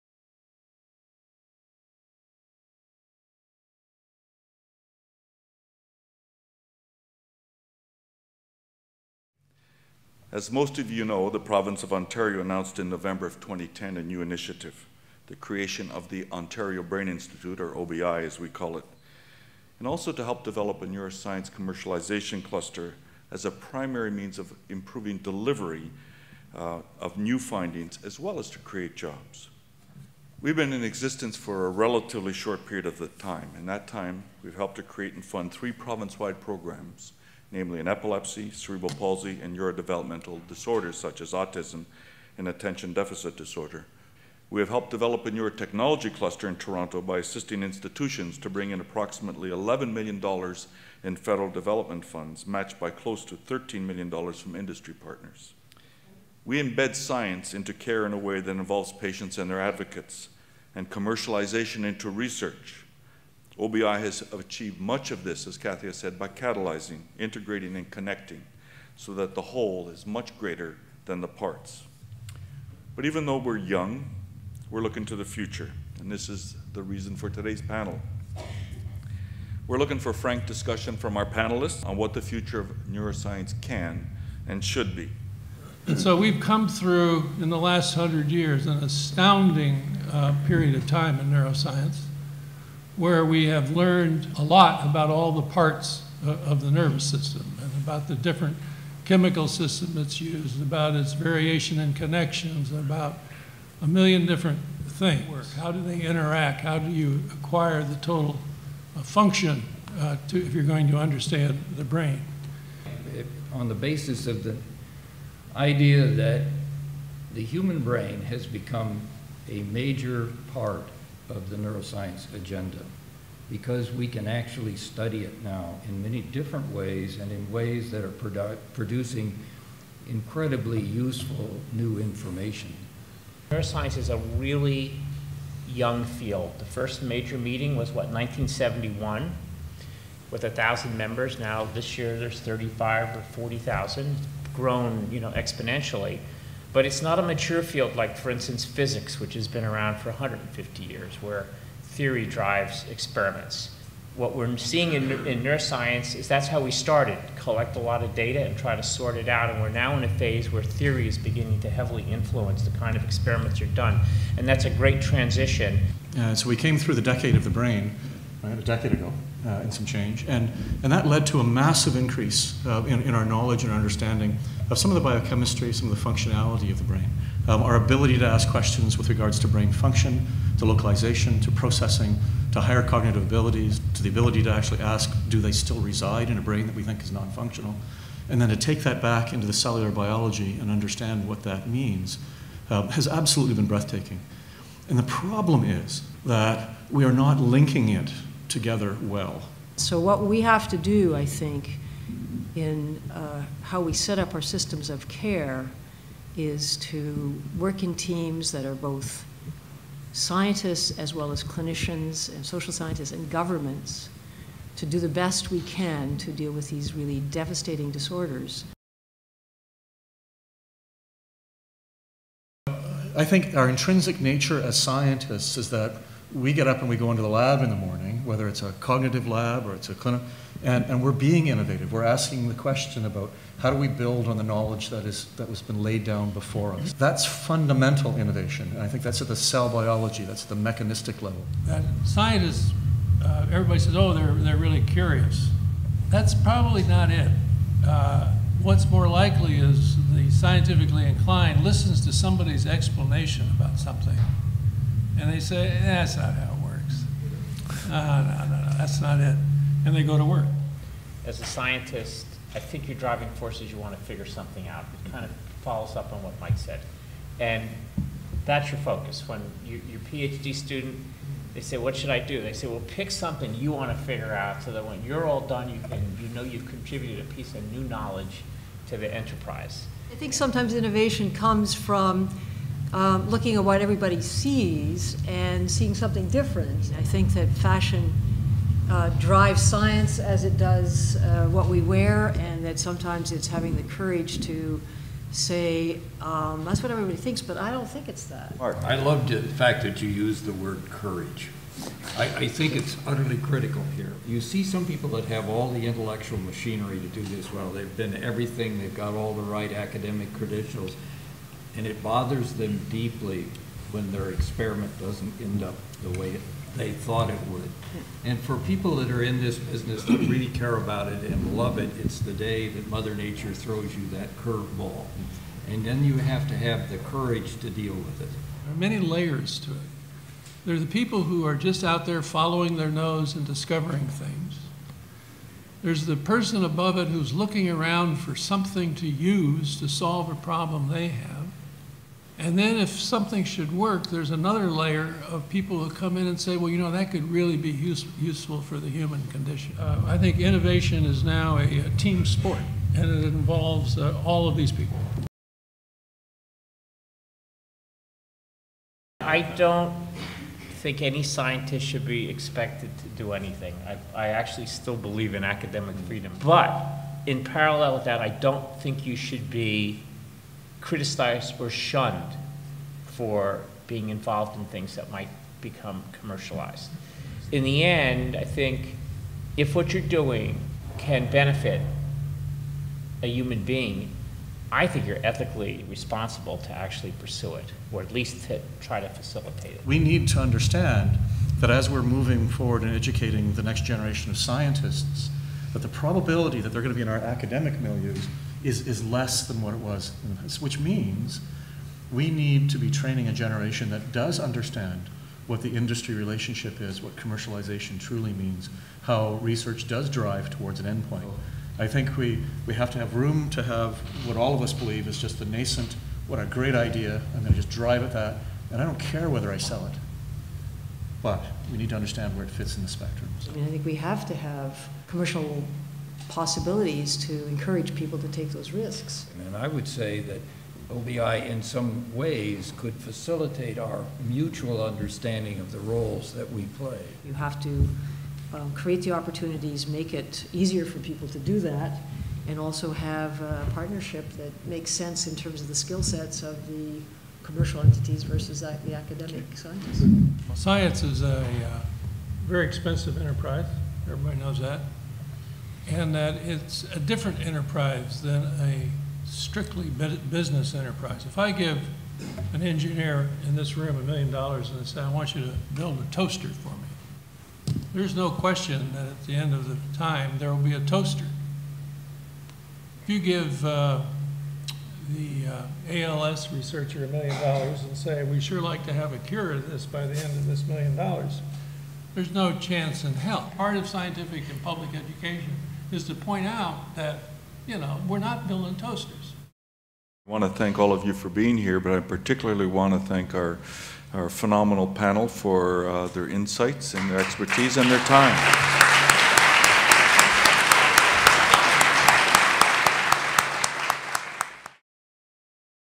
Integrating Brain Research: Basic Science To Innovation
A Scientific Panel discussion chaired by Dr. Donald T. Stuss, President and Scientific Director of the Ontario Brain Institute was held on November 27th, 2012 at the Royal Ontario Museum, Eaton Theatre. This video captures some of the highlights from the panel discussion between Dr. Michael S. Gazzaniga, Dr. Robert T. Knight, Dr. Michael Strong, Dr. Marcus Raichle and Dr. Sandra Black